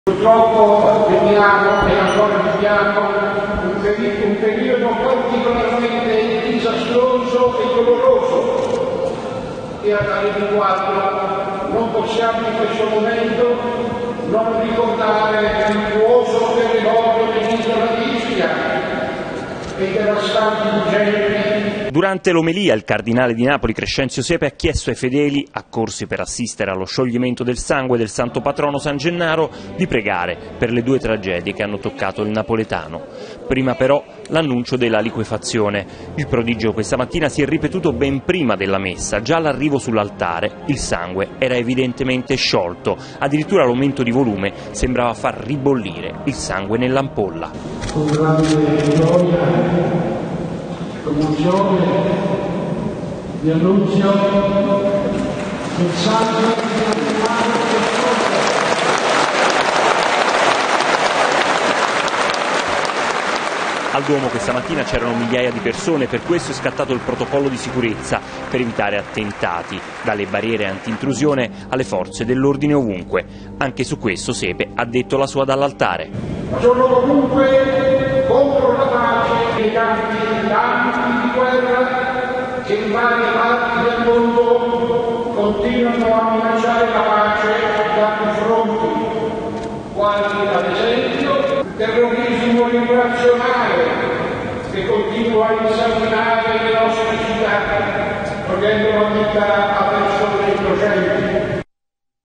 Napoli in festa: si ripete il miracolo di San Gennaro
Napoli si ferma per San Gennaro. Si è ripetuto il miracolo del sangue. Lo ha annunciato il cardinale Sepe. Miracolo che è segno di buon auspicio per il capoluogo campano e per tutta la regione. Servizio di Marco Caiano